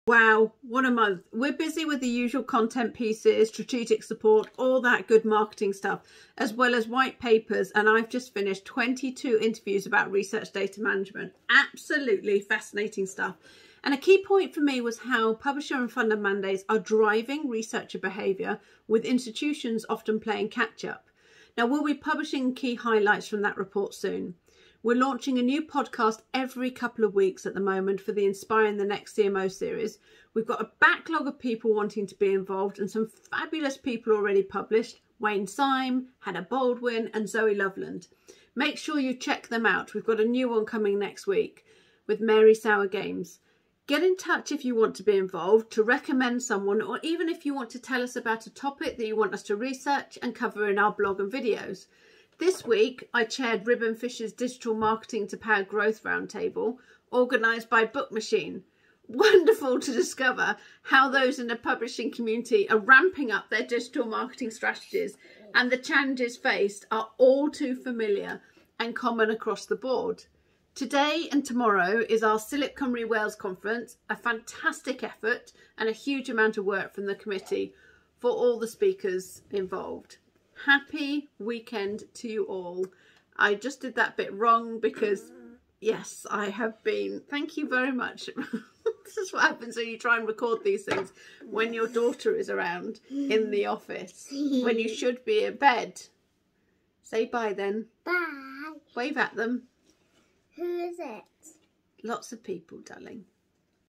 May newsletter 2021
You know when they say - don't work with animals and children... my daughter makes an appearance at the end of this. I debated about doing another take and then decided...no this is working life in all its glory!

Wow, what a month! We're busy with the usual content pieces, strategic support, as well as white papers and I've just finished 22 interviews about research data management - fascinating stuff and a key point for me was how publisher and funder mandates are driving researcher behaviour, with institutions often playing catch up. We'll be publishing key highlights from that report soon.

We're launching a new podcast every couple of weeks at the moment for the Inspiring the Next CMO series. We've got a backlog of people wanting to be involved and some fabulous people already published - Wayne Sime, Hannah Baldwin and Zoe Loveland  - make sure you check them out. Get in touch if you want to be involved, to recommend someone or even just tell us a topic for us to research and cover in our blog and videos.

This week I chaired Ribbonfish's Digital Marketing to Power Growth roundtable, organised by BookMachine. Wonderful to discover how those in the publishing community are ramping up their digital marketing strategies and the challenges faced are all too familiar and common across the board. Today and tomorrow is our CILIP Cymru Wales conference, a fantastic effort and a huge amount of work from the committee, and all the speakers involved.

Happy weekend to you all.

Lou